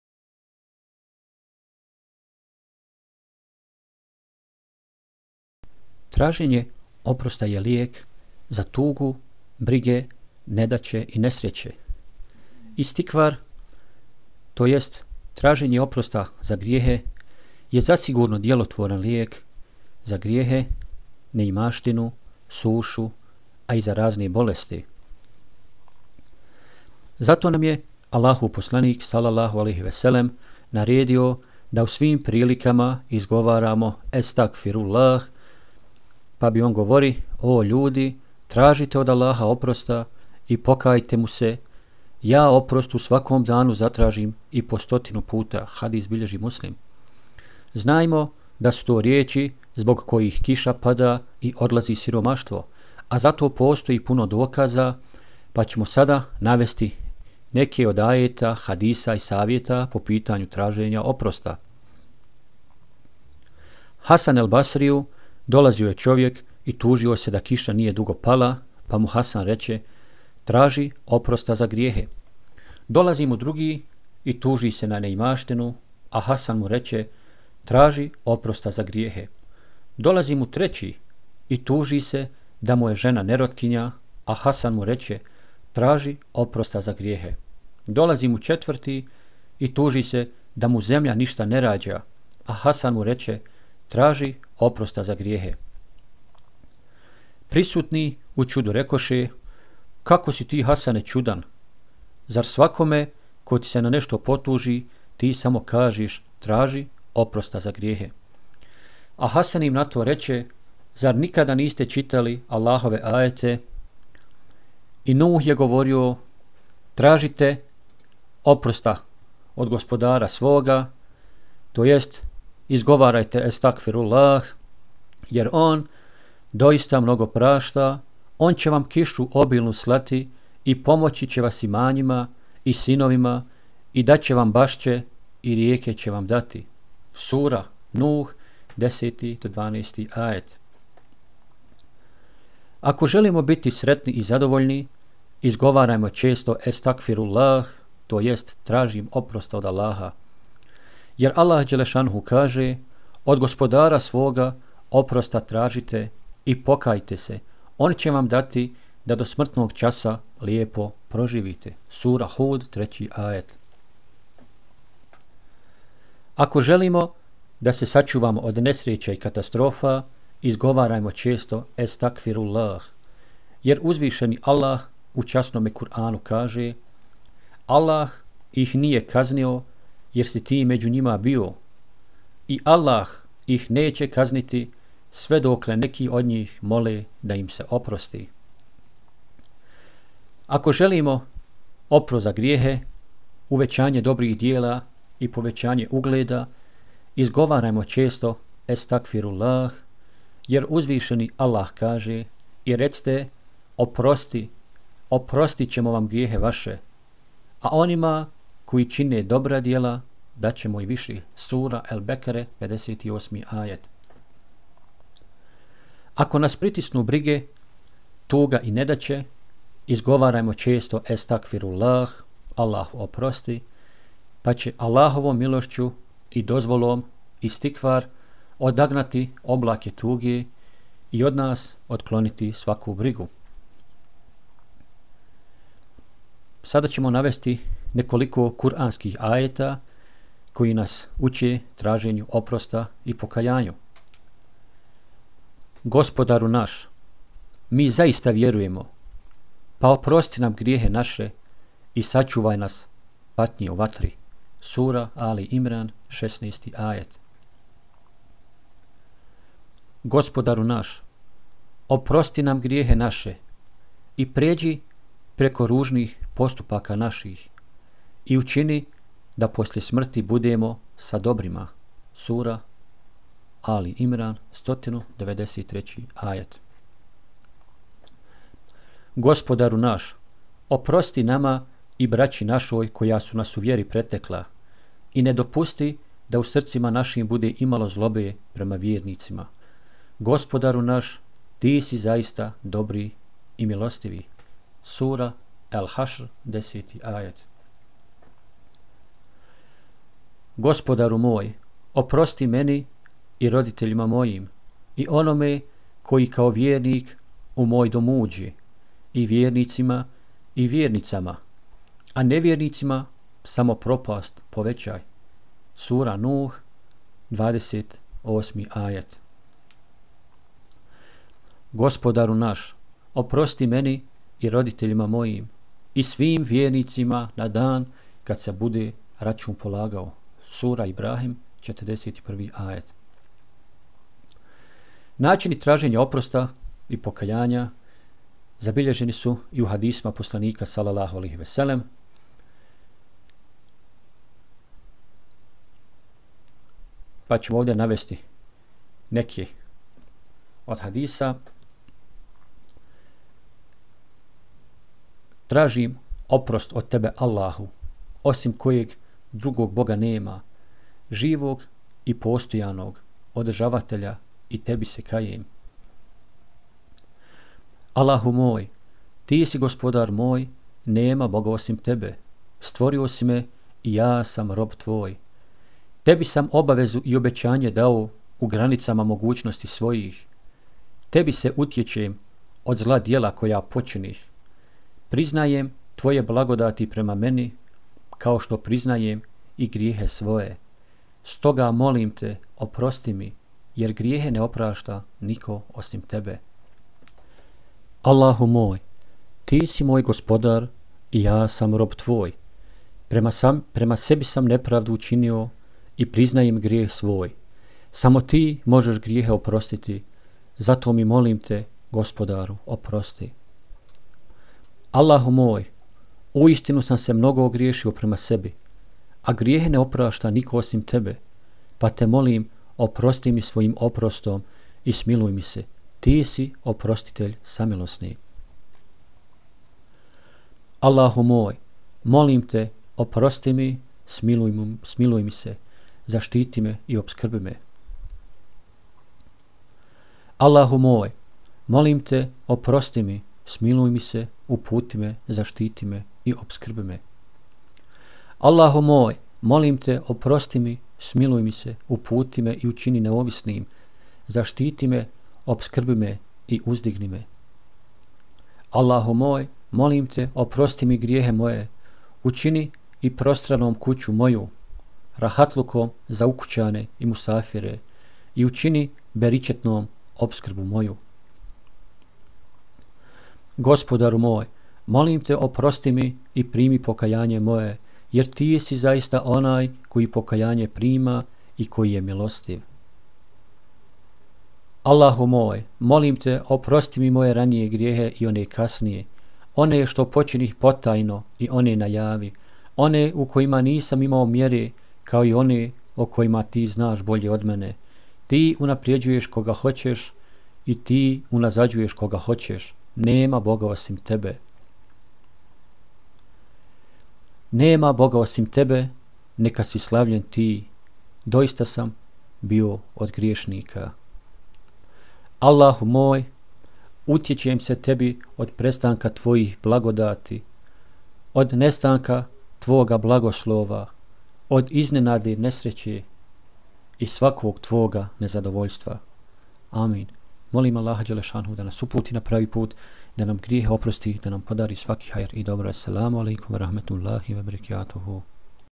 ESTAGFIRULLAH - TRAŽENJE OPROSTA JE LIJEK ZA TUGU, BRIGE, NEDAĆE I RAZNE BOLESTI
Estagfirullah -  Allahu oprosti

Traženje oprosta je lijek za tugu, brige, nedaće i nesreće

Istigfār (traženje oprosta za grijehe) je zasigurno djelotvoran lijek za grijehe, neimaštinu, sušu, a i za razne bolesti. Zato nam je Allahov Poslanik, a.s., naredio da u svim prilikama izgovaramo: "Estagfirullah". Govorio je, a.s.: „O ljudi, tražite od Allaha oprosta i pokajte Mu se (tewbu činite). Ja oprost u svakom danu zatražim i po 100 puta.“ (Muslim). Znajmo da su to riječi zbog kojih kiša pada i odlazi siromaštvo a za to postoji puno dokaza pa pročitajte par korisnih ajeta, hadisa i savjeta.

Hasan el-Basriju dolazio je čovjek i tužio se da kiša nije dugo pala, pa mu Hasan reče: „Traži oprosta za grijehe.“ Dolazi mu drugi i tuži se na neimaštinu, a Hasan mu reče: „Traži oprosta za grijehe.“ Dolazi mu treći i tuži se da mu je žena nerotkinja, a
Hasan mu reče: „Traži oprosta za grijehe.“ Dolazi mu četvrti i tuži se da mu zemlja ništa ne rađa, a Hasan mu reče: „Traži oprosta za grijehe.“ Prisutni u čudu rekoše: „Kako si ti Hasane čudan! Zar svakome ko ti se na nešto potuži ti samo kažeš: 'Traži oprosta za grijehe?‘…“
A Hasan im na to reče: „Zar nikada niste čitali Allahove ajete: „...i (Nuh je) govorio: 'Tražite od Gospodara svoga oprost (estagfirullah) jer On, doista, mnogo prašta; On će vam kišu obilnu slati i pomoći će vas imanjima i sinovima, i daće vam bašće, i rijeke će vam dati.“ (Nuh, 10-12)
Ako želimo biti srećni i zadovoljni, izgovarajmo često: estagfirullah (tražim oprost od Allaha), jer Allah, dž.š., kaže: „... od Gospodara svoga oprosta tražite i pokajte se, On će vam dati da do smrtnog časa lijepo proživite…“ (Hud, 3)

Ako želimo da se sačuvamo od nesreća i katastrofa, izgovarajmo često: estagfirullah (tražim oprost od Allaha), jer Uzvišeni Allah u Časnome Kur'anu kaže: „Allah ih nije kaznio, jer si ti među njima bio; i Allah ih neće kazniti sve dokle neki od njih mole da im se oprosti.“ (El-Enfal, 3)

Ako želimo oprost za grijehe, uvećanje dobrih djela i povećanje ugleda, izgovarajmo često: estagfirullah (tražim oprost od Allaha), jer Uzvišeni kaže: „... i recite: 'Oprosti' – oprostićemo vam grijehe vaše, a onima koji čine dobra djela daćemo i više!“ (El-Bekara, 58)

Ako nas pritisnu brige, tuga i nedaće, izgovarajmo često: estagfirullah (tražim oprost od Allaha), pa će Allahovom milošću i dozvolom istigfār odagnati oblake tuge i od tebe otklonuti svaku brigu!